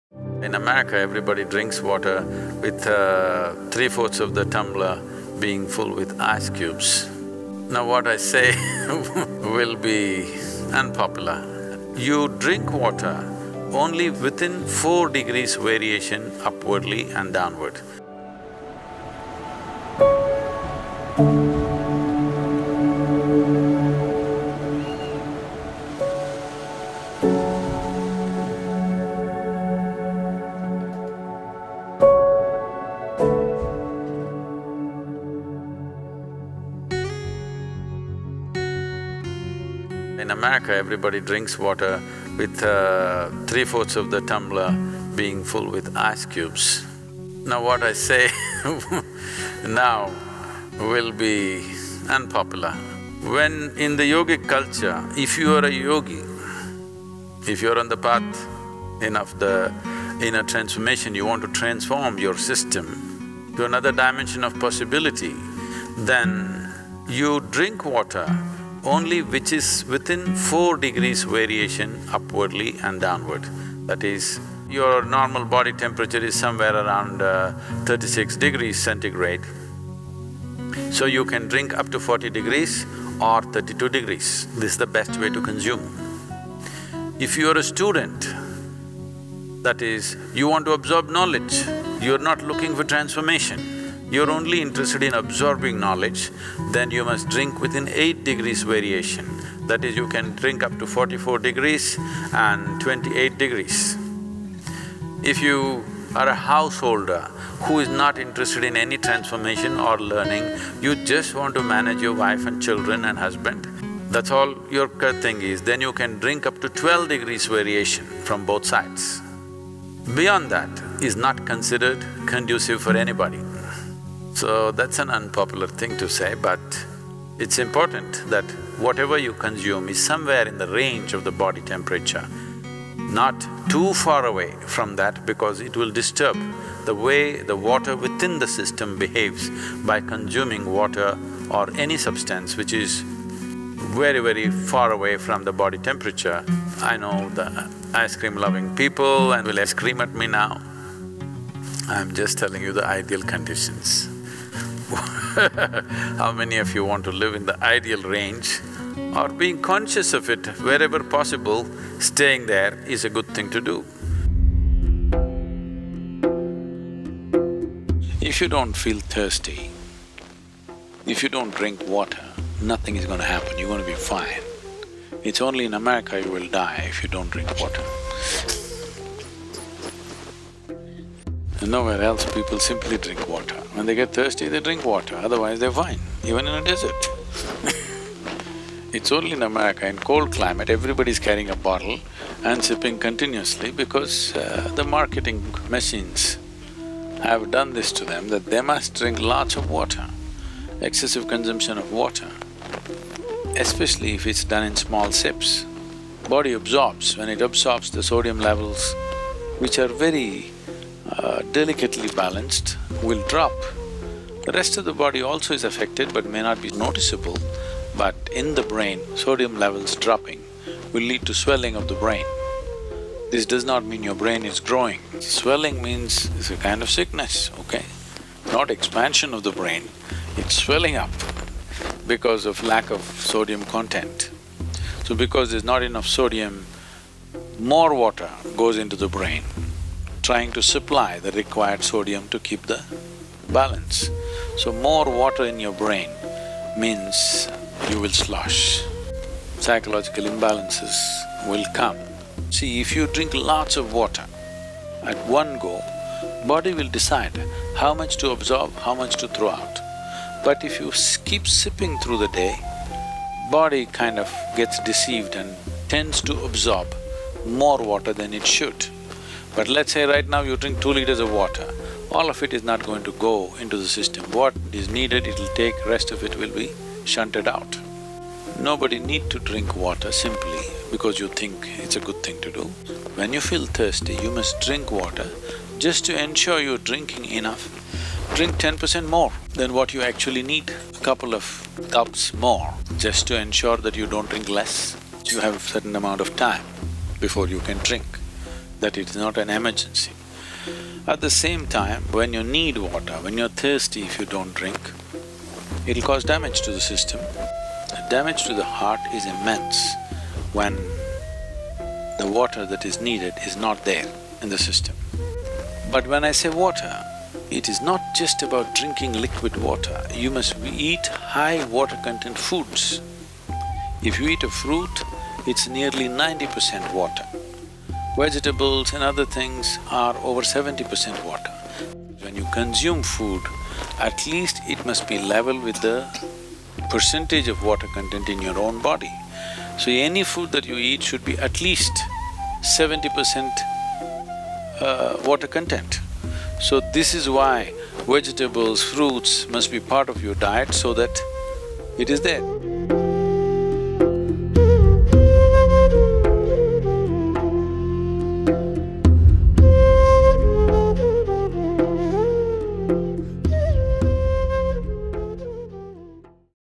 Are You Drinking Water the Right Way? - Sadhguru
In this video, Sadhguru talks about the right way to drink water, cautioning that improper ways of drinking water can lead to brain swelling.